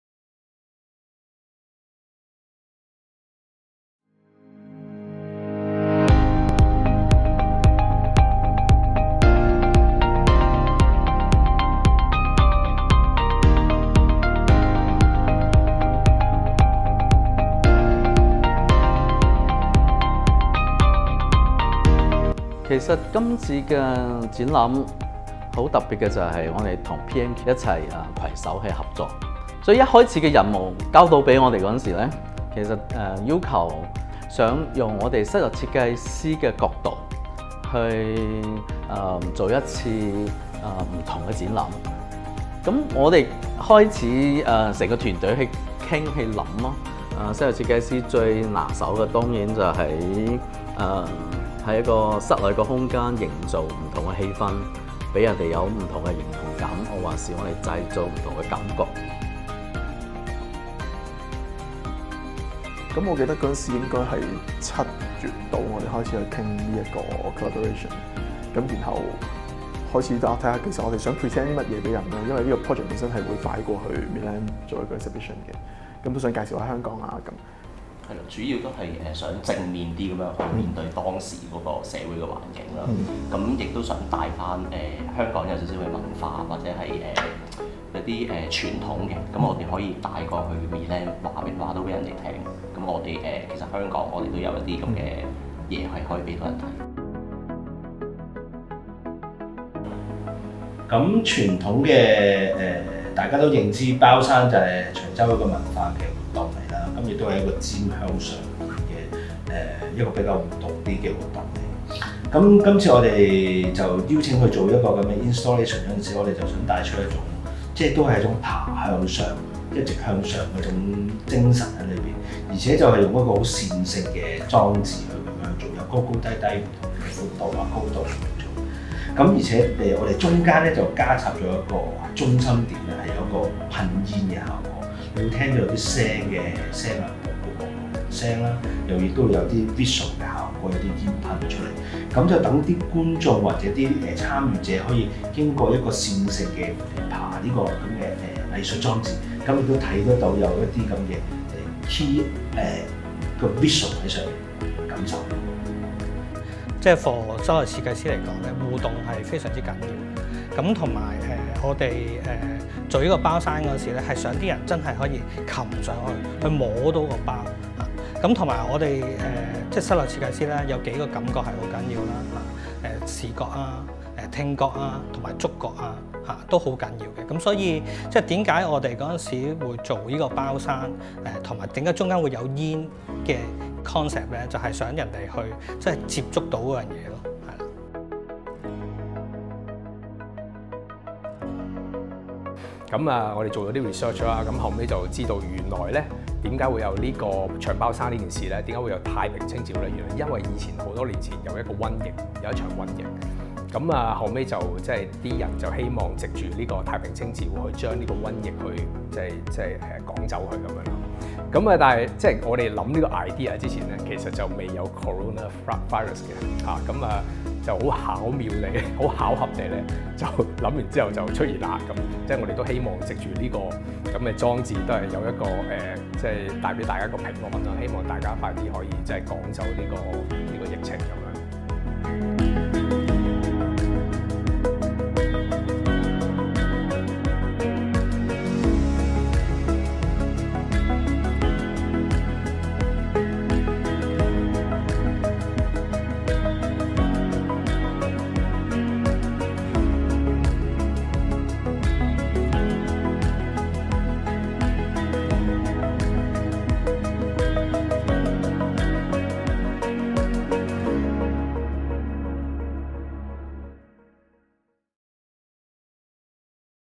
Re-edit Hong Kong Showcase 香港設計展現｜Hong Kong Interior Design Association “Bao Shan” 香港室內設計協會 《包山》
從傳統文化中創出新意 呈現《包山》不屈不撓的精神

Re-edit 香港設計展現大型藝術裝置作品《包山》由香港室內設計協會的設計師何宗憲、許諾、陳狄康、林子康、鄒卓明、張敬貴攜手打造。《包山》的靈感取自於長洲的太平清醮的「包山」。每年太平清醮，數以百計的參賽健兒爭相攀上掛滿平安包、高十四米，稱為「包山」的圓錐形竹塔，爭奪塔尖上的平安包，藉此祈求神靈驅趕病魔與惡運。《包山》既體現太平清醮的精神，亦探討了如何從傳統文化中創出新意。此作品用了九千個平安包組成山巒起伏的地景，配以燈光設計及煙霧機裝置，將《包山》重塑與引申成為香港的文化工藝，希望令觀眾感受這個廣為人知的香港風俗，並欣賞作品所呈現的不屈不撓精神。

Re-edit香港設計展現
日期：2020年9月18日至10月4日
時間：上午11時至下午 8時
地點：PMQ 元創方地面廣場
免費入場

主要贊助機構：香港特別行政區政府「創意香港」

REMAKING NEW IDEA FROM CELEBRATED HERITAGE AND APPRECIATEING THE RESILLIENCE REPRESENTED BY “BAO SHAN”

 “Bao Shan”, the large-scale art installation of Re-edit Hong Kong Showcase, is designed by members of Hong Kong Interior Design Association, Joey Ho, Enoch Hui, Tik Chan, Max Lam, CM Jao, Ken Cheung.  It is inspired by Cheung Chau Tai Ping Purification Rites, as known as the Bun Festival.  Every year, hundreds of daredevils on Cheung Chau Island race up 14-metre-high conical bamboo towers covered in steamed buns to snatch the top bun.  It is believed that these buns, each stamped in food colouring with the characters for safety and peace, bring blessings to those who are able to grab them. “Bao Shan” encapsulate the spirit of the Bun Festival and explore how a celebrated heritage can be remade into something new. Composed of a more approachable set of rolling hills, and fitted with lighting fixtures and a fog machine, the 9,000-bun installation playfully reimagines the bun mountains as a metonymy of actual cultural artefacts found in Hong Kong. Visitors can connect with this popular Hong Kong tradition and appreciate the spirit of resilience conveyed by this unusual work.

Re-edit Hong Kong Showcase
Date:  18 September to 4 October, 2020
Time:  11am to 8pm
Venue：Marketplace, PMQ
Free entry